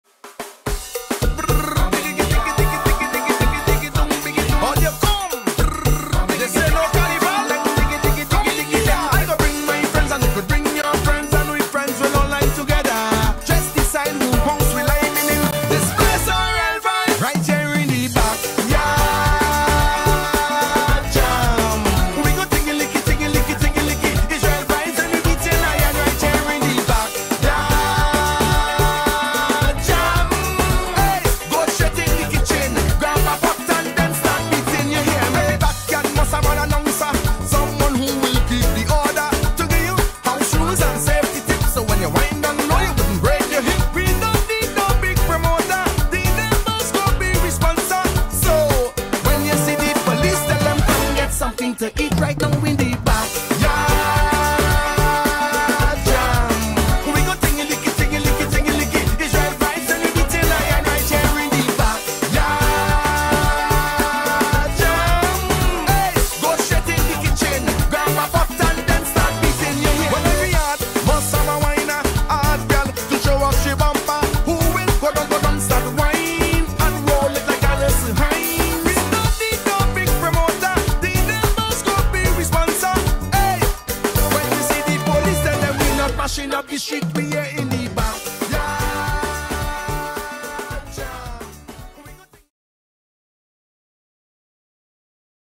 New York
Labor Day weekend Backyard jam was NICE!!! I can’t wait to return next year.